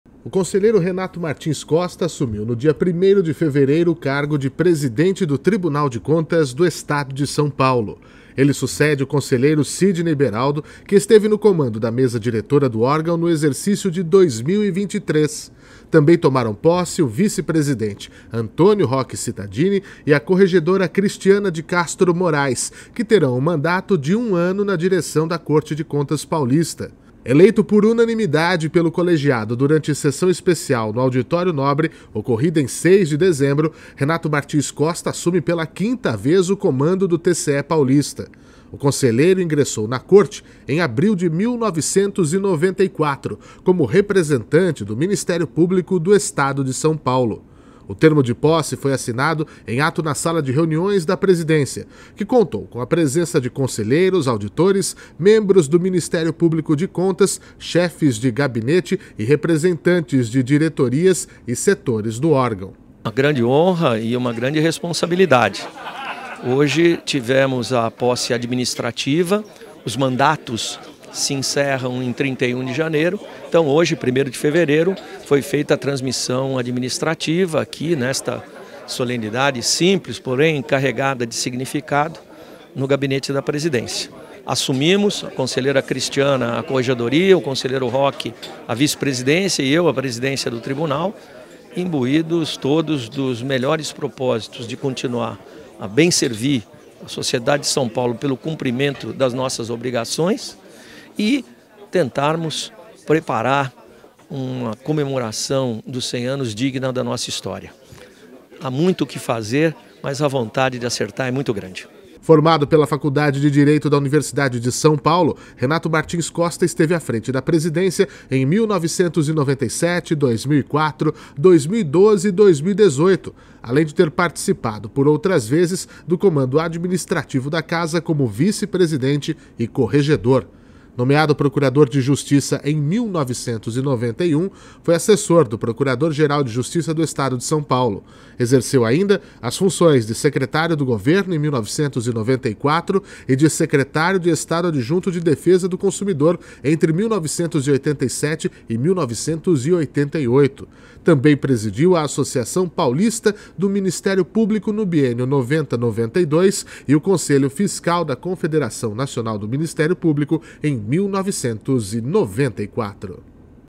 Renato Martins Costa assume a Presidência do TCE no ano do Centenário
O Conselheiro Renato Martins Costa assumiu no dia 1º de fevereiro o cargo de Presidente do Tribunal de Contas do Estado de São Paulo.

Ele sucede o Conselheiro Sidney Beraldo, que esteve no comando da Mesa Diretora do órgão no exercício de 2023.

Também tomaram posse o Vice-Presidente Antonio Roque Citadini e a Corregedora Cristiana de Castro Moraes, que terão o mandato de um ano na direção da Corte de Contas paulista.

Eleito por unanimidade pelo Colegiado durante sessão especial no Auditório Nobre ocorrida em 6 de dezembro, Renato Martins Costa assume pela quinta vez o comando do TCE paulista. O Conselheiro ingressou na Corte em abril de 1994, como representante do Ministério Público do Estado de São Paulo.

O termo de posse foi assinado em ato na sala de reuniões da Presidência, que contou com a presença de Conselheiros, Auditores, Membros do Ministério Público de Contas, Chefes de Gabinete e representantes de Diretorias e setores do órgão.

Formado pela Faculdade de Direito da Universidade de São Paulo, Renato Martins Costa esteve à frente da Presidência em 1997, 2004, 2012 e 2018, além de ter participado, por outras vezes, do comando administrativo da Casa como Vice-Presidente e Corregedor.

Nomeado Procurador de Justiça em 1991, foi assessor do Procurador-Geral de Justiça do Estado de São Paulo. Exerceu ainda as funções de Secretário do Governo (1994) e de Secretário de Estado Adjunto de Defesa do Consumidor (1987/1988). Também presidiu a Associação Paulista do Ministério Público no biênio 1990/1992 e o Conselho Fiscal da Confederação Nacional do Ministério Público em 1994.